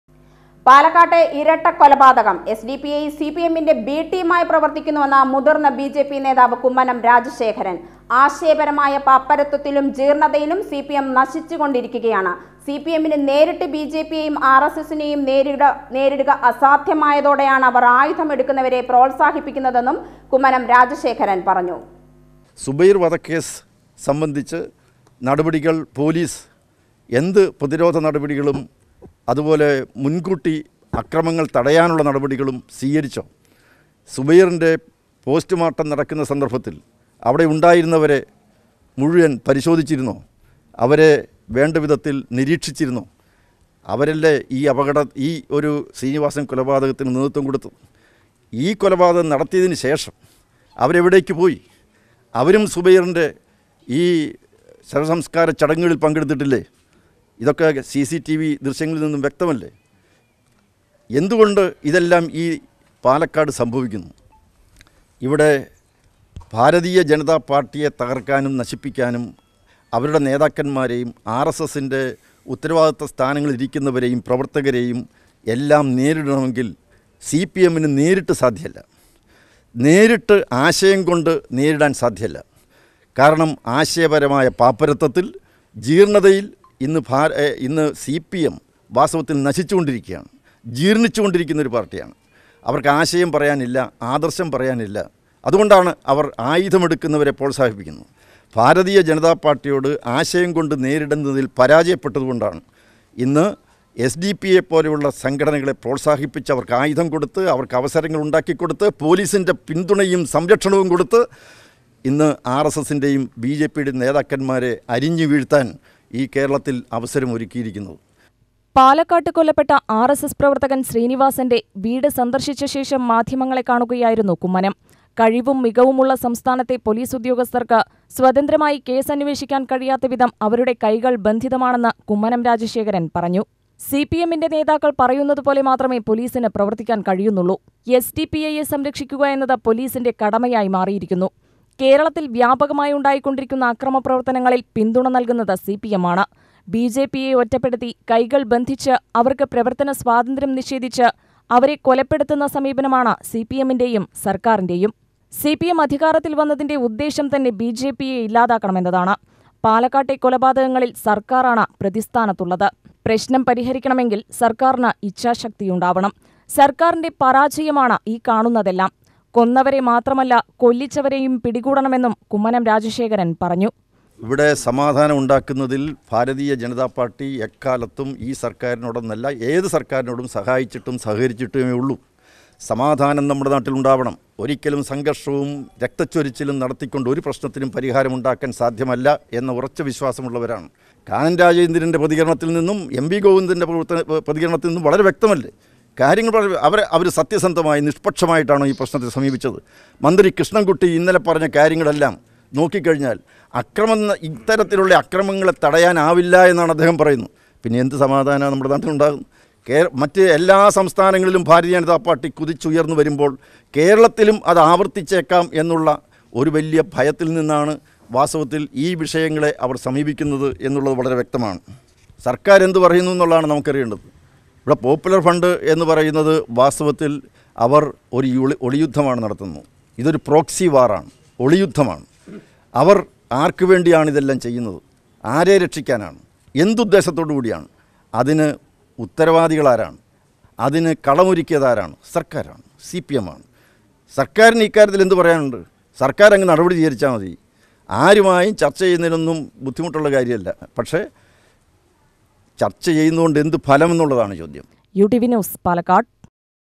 എസ് ഡി പി ഐ, സിപിഎമ്മിന്റെ ബി ടീം ആയി പ്രവർത്തിക്കുന്നുവെന്ന് മുതിർന്ന ബിജെപി നേതാവ് കുമ്മനംരാജശേഖരൻ
പാലക്കാട്ടെ ഇരട്ട കൊലപാതകം : എസ് ഡി പി ഐ, സി പി എമ്മിന്റെ ബി ടീം ആയി പ്രവർത്തിക്കുന്നുവെന്ന് മുതിർന്ന ബി ജെ പി നേതാവ് കുമ്മനം രാജ ശേഖരൻ. ആശയപരമായ പാപ്പരത്വത്തിലും ജീർണതയിലും സി പി എം നശിച്ചു കൊണ്ടിരിക്കുകയാണ്. സി പി എമ്മിന് നേരിട്ട് ബി ജെ പിയെയും ആർ എസ് എസിനെയും നേരിടുക അസാധ്യമായതോടെയാണ് അവർ ആയുധം എടുക്കുന്നവരെ പ്രോത്സാഹിപ്പിക്കുന്നതെന്നും കുമ്മനം രാജ ശേഖരൻ പറഞ്ഞു.